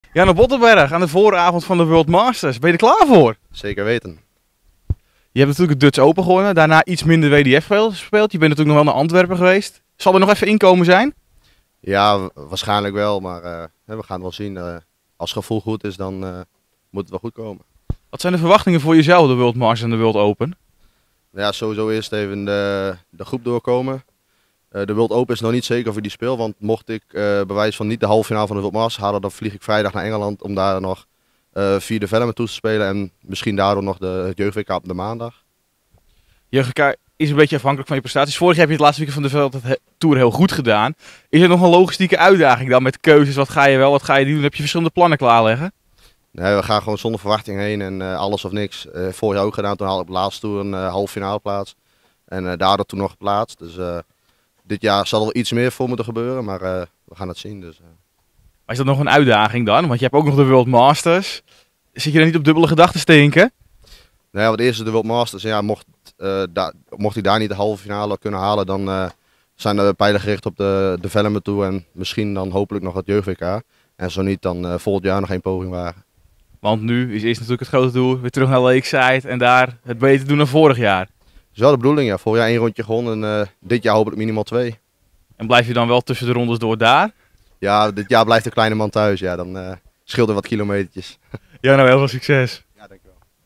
Jarno Bottenberg: 'Als ik niet de zaterdag haal van de World Masters, ga ik nog voor het Jeugd WK'
Jarno Bottenberg, de winnaar van de TOTO Dutch Open Darts 2024, was afgelopen weekend aanwezig op het Open Assen. De Fries bereikte de laatste 32, waarin hij het uiteindelijk moest afleggen tegen Dennis Gebben met 3-1. Bottenberg heeft een drukke week voor de boeg met de WDF World Masters, de WDF World Open, de Development Tour en eventueel het PDC Jeugd WK op het programma. Darts Actueel sprak met 'Bottie' over zijn plannen en verwachtingen voor de komende week.

Al het dartnieuws, waaronder nieuws over de WDF World Masters, is terug te vinden op onze website!